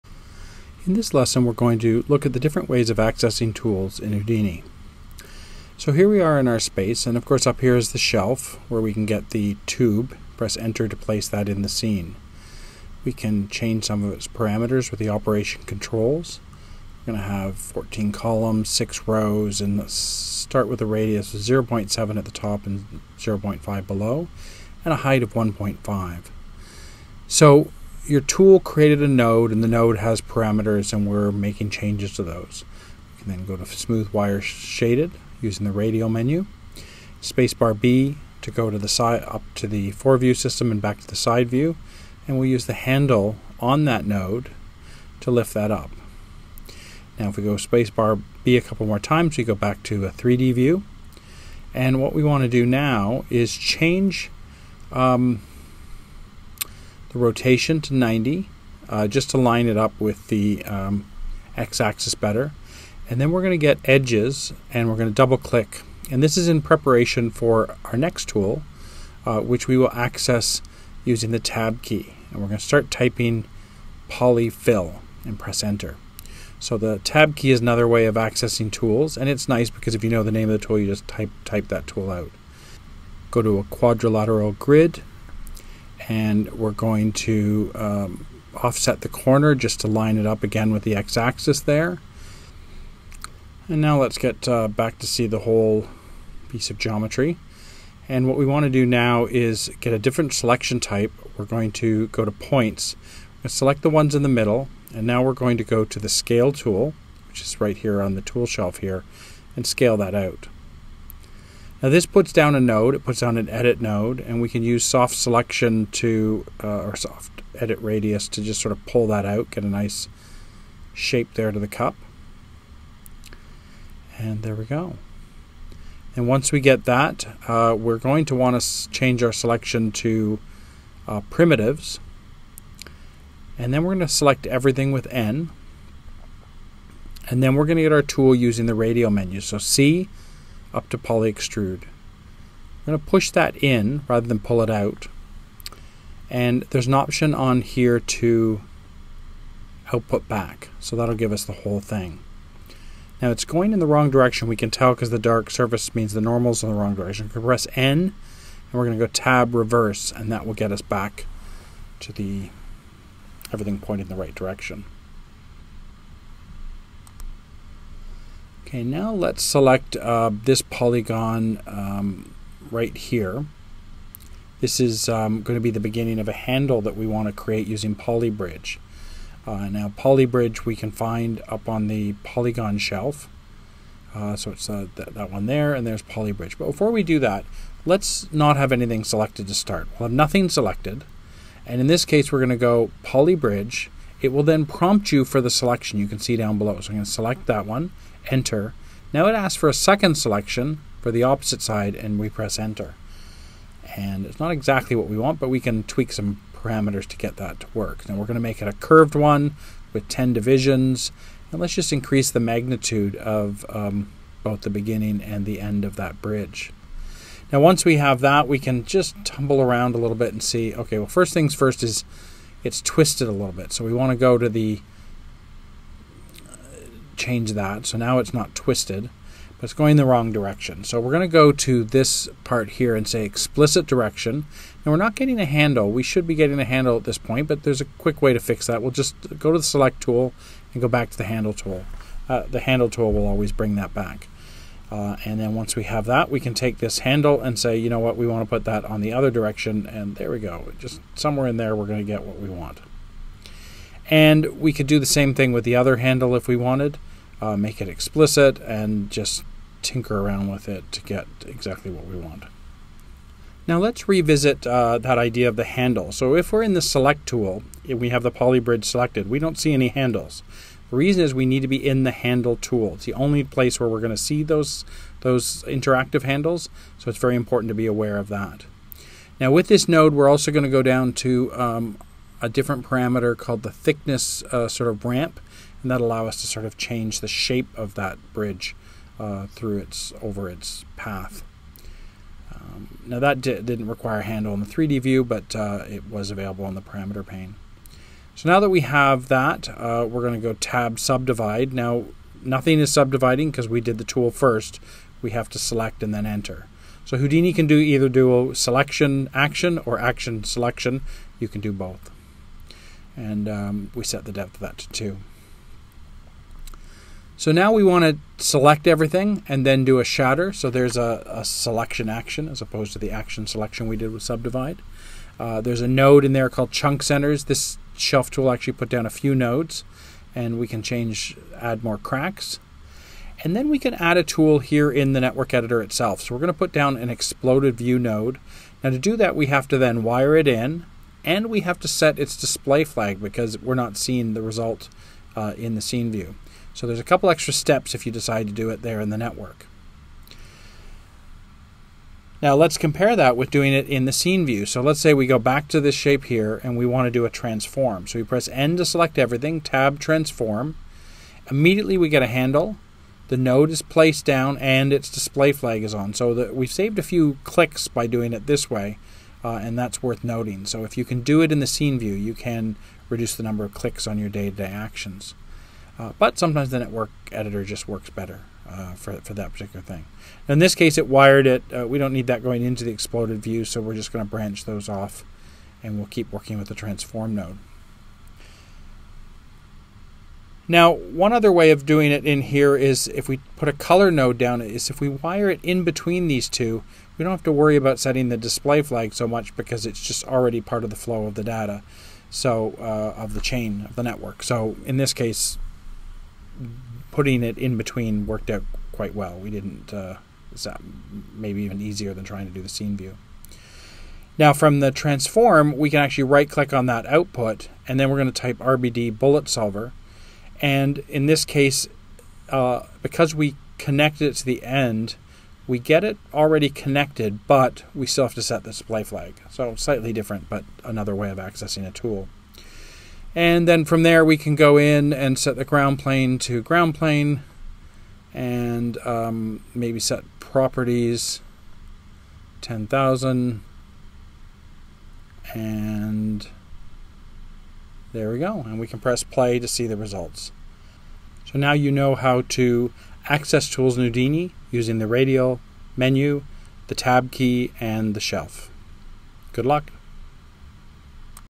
Houdini Foundations | Overview 9 | Accessing Tools
Learn how to access tools using the Shelf, Radial menus and the tab key. Learn how to use tools in the scene view or add nodes to the network editor to build the networks you need for your work.